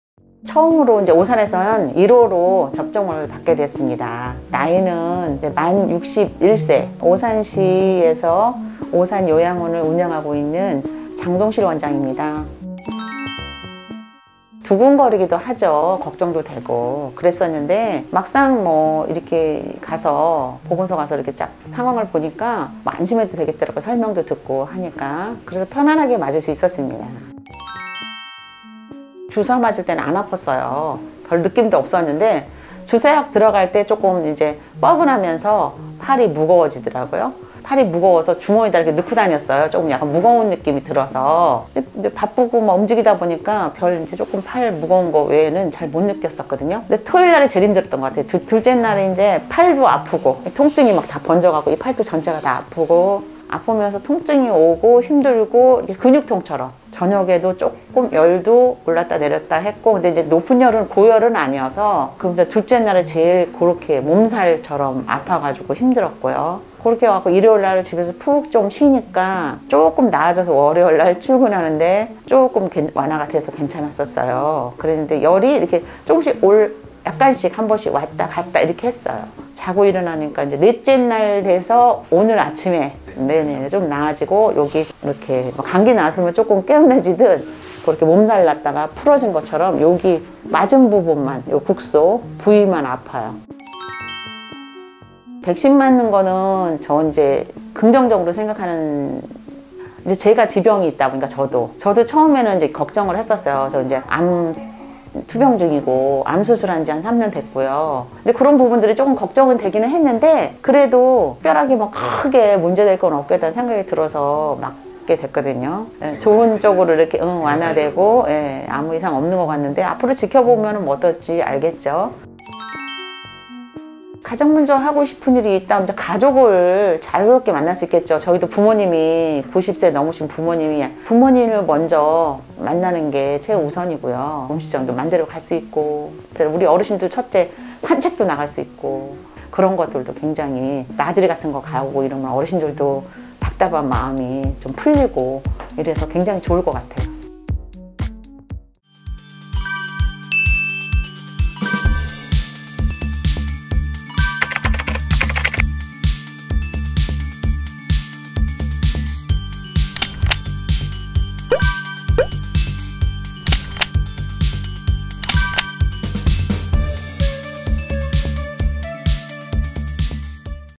※코로나 백신 1호 접종자가 말하는 찐후기※
오산에서 가장 먼저 코로나 백신 맞은 오산요양원 장동실원장!
"백신 맞고 2일째 되는 날은 근육통이랑 몸살이 났어요"